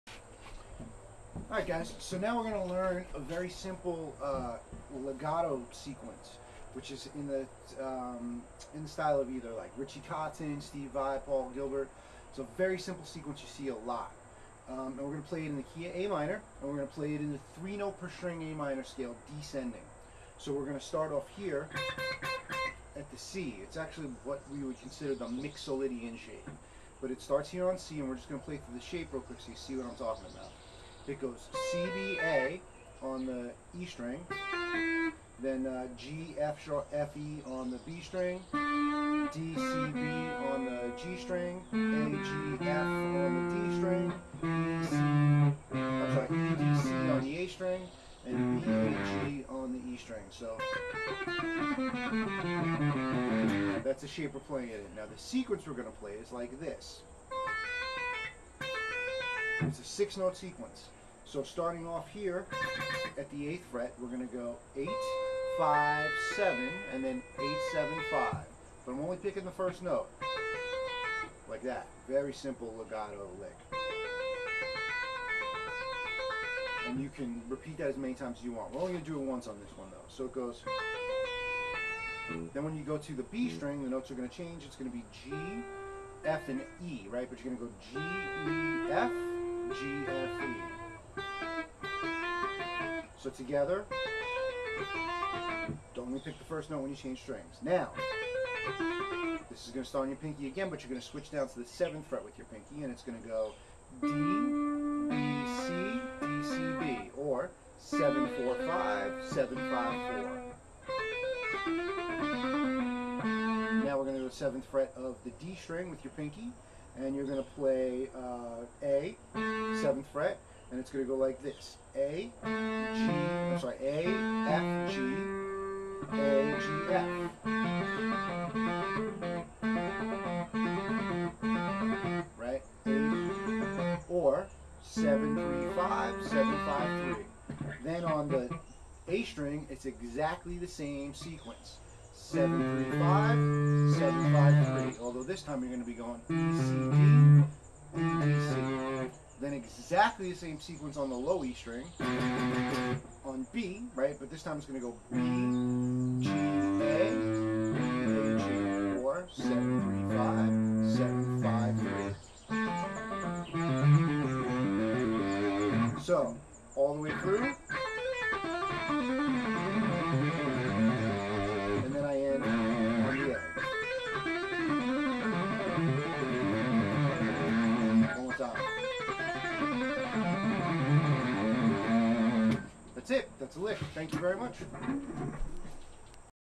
Joe Buddha's Quick Lick week # 3. Descending Legato run in A minor.
This is a great little run that repeats a 6 note sequence in A minor.
It is a pretty common sequence used by Steve Vai, Richie Kotzen, Paul Gilbert and Joe Satriani.
 Only pick the first note on each string.
Happy Shredding!!